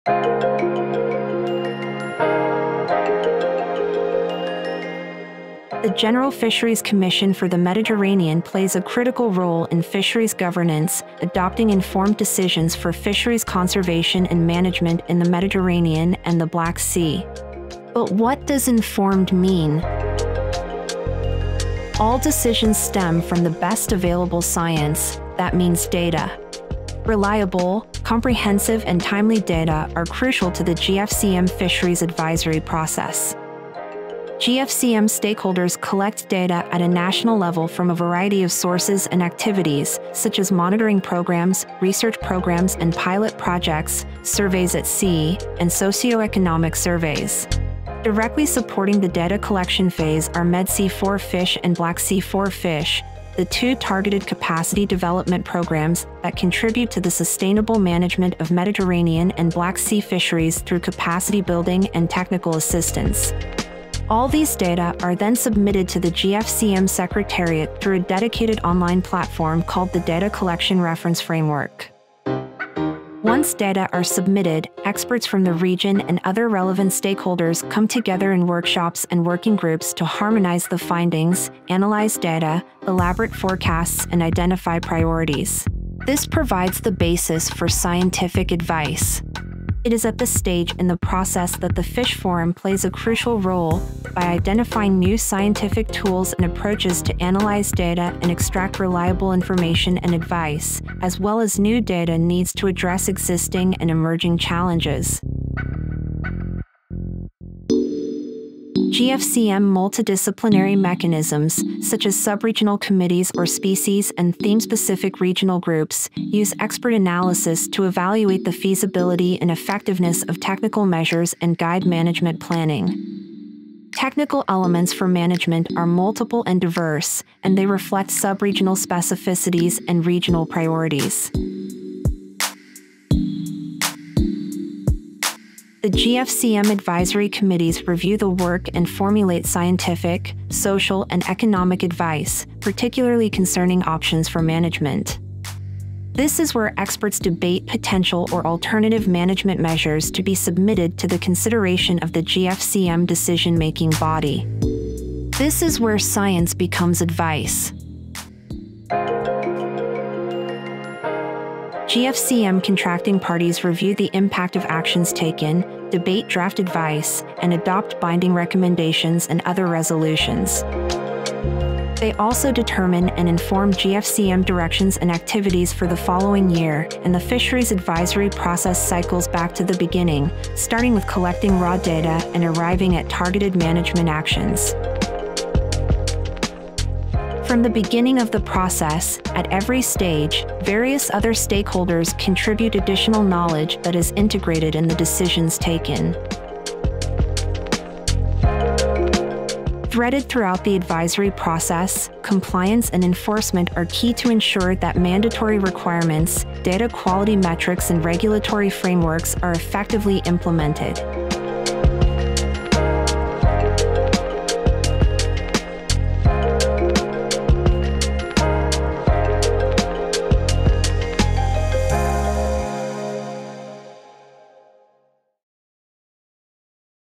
The GFCM Fisheries advisory process: from raw data to targeted management
Fisheries

The General Fisheries Commission for the Mediterranean (GFCM) plays a critical role in fisheries governance, adopting informed decisions for fisheries conservation and management in the Mediterranean and the Black Sea.

But what does “informed” mean?

This video explains in detail the GFCM fisheries advisory process, providing insights into every stage, starting from collecting raw data and arriving at targeted management actions.